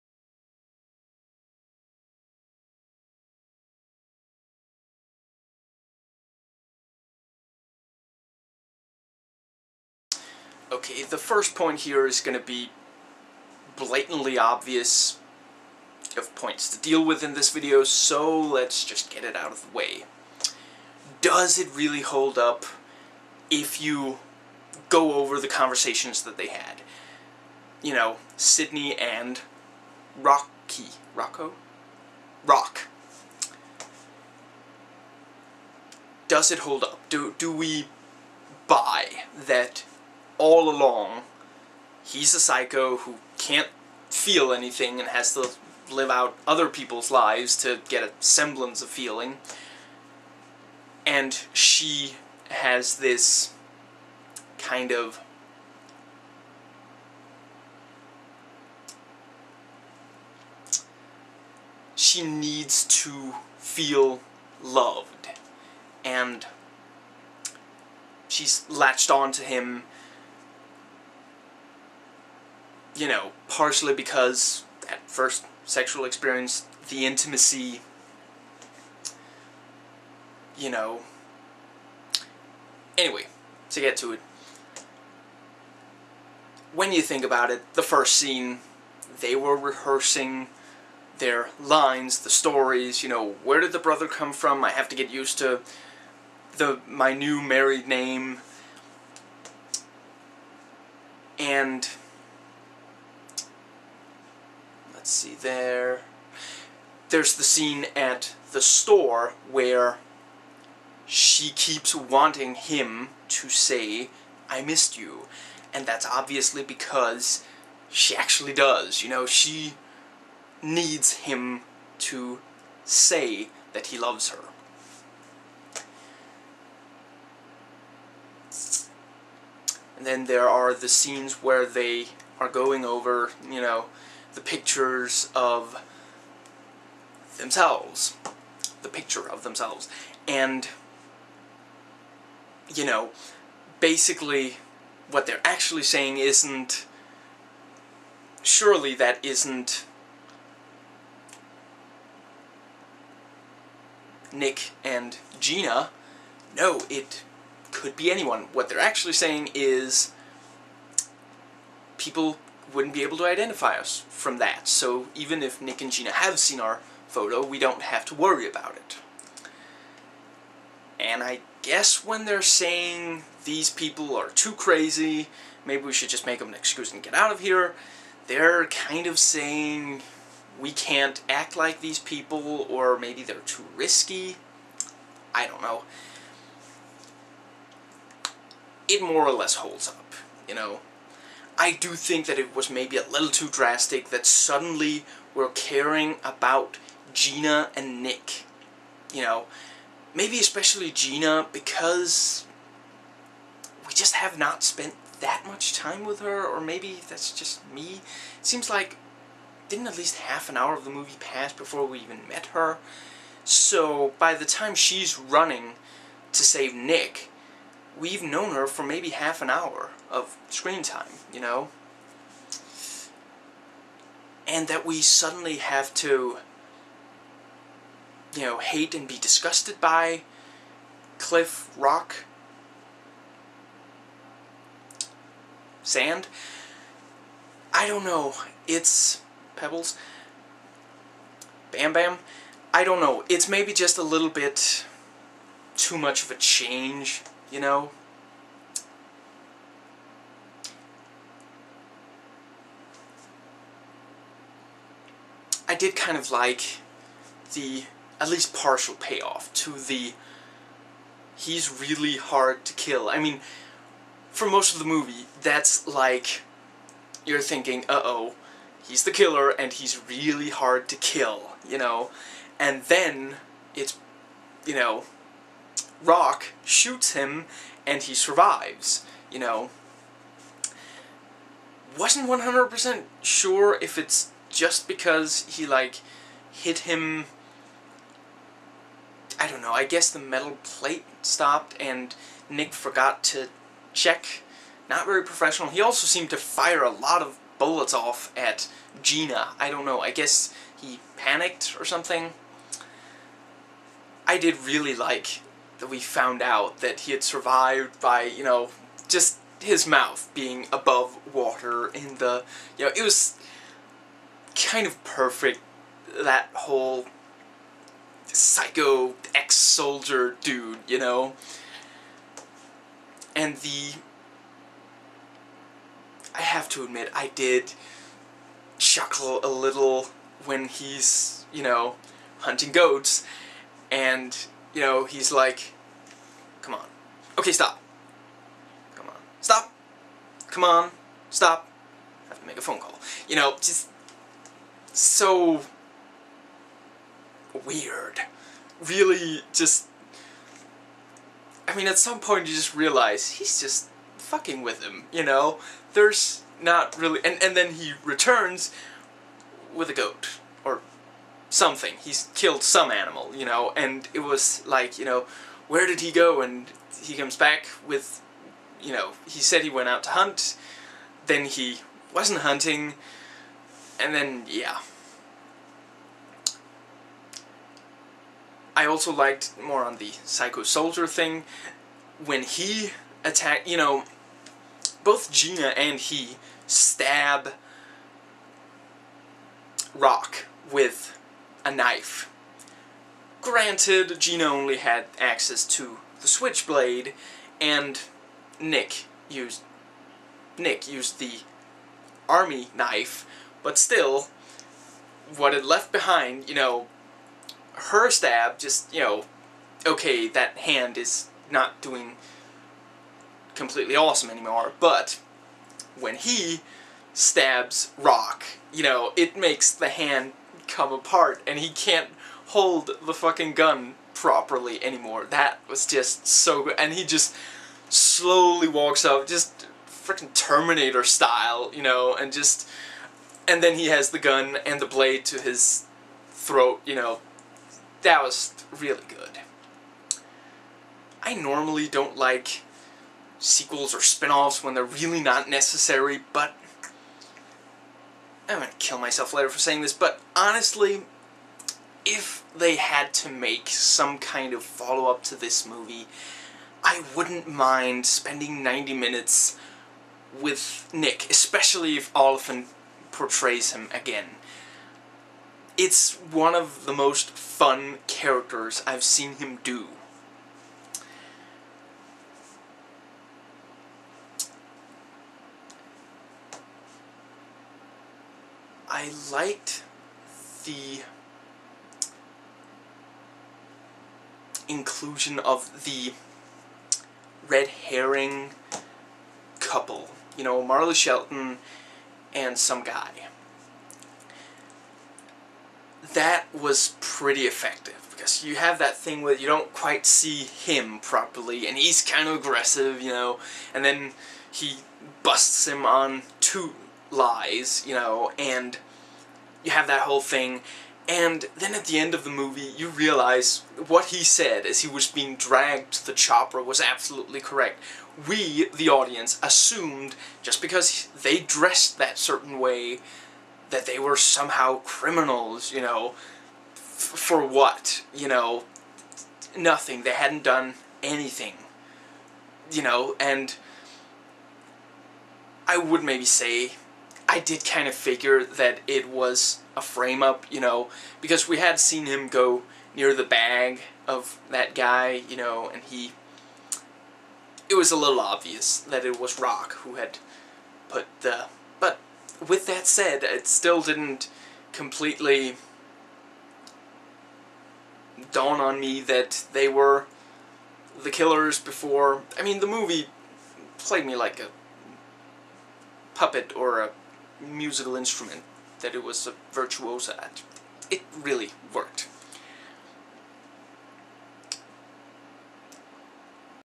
1/2 Thoughts: A Perfect Getaway
Spoiler alert: I reveal a lot of what happens in the movie, including details of the ending; I made this mainly for those who have already watched it. This is where I get into specific stuff of the film. I swear a little in this, for anyone bothered by such.

If you want a spoiler-free review of it, I have also recorded one, and you can find it

I recorded this today after having watched it. I hope you enjoy it, and that I can help spark debate about the film.

This is a pure one-taker, so occasionally, I misspeak or begin a sentence without ending it. If you either really prefer this approach for videos or prefer me to edit them, *please* post your opinion, and I will see which is preferred.